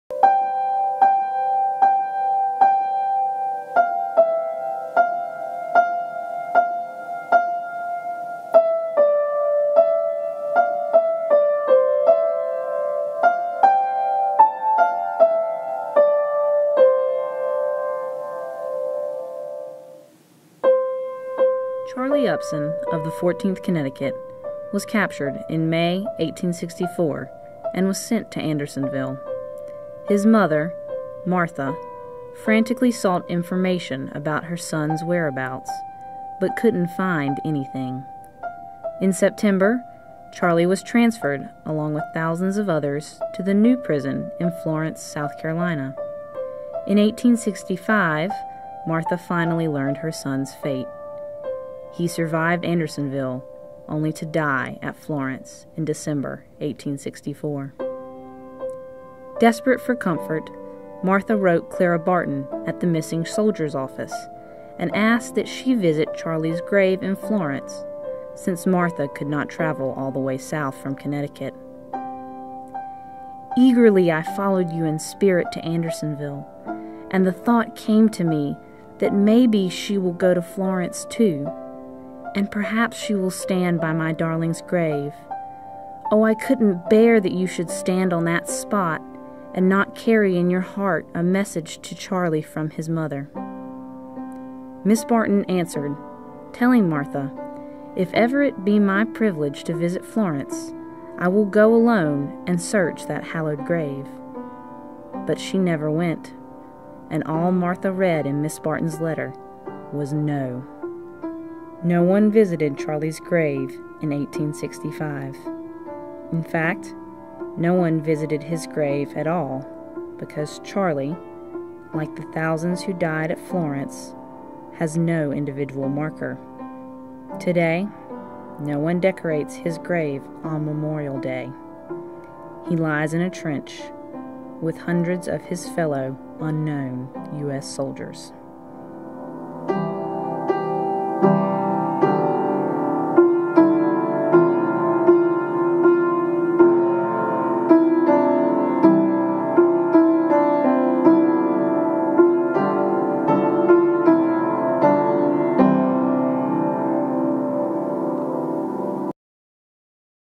Charlie Upson: A Story in Stone
Charlie Upson's mother searched in vain for her son's grave.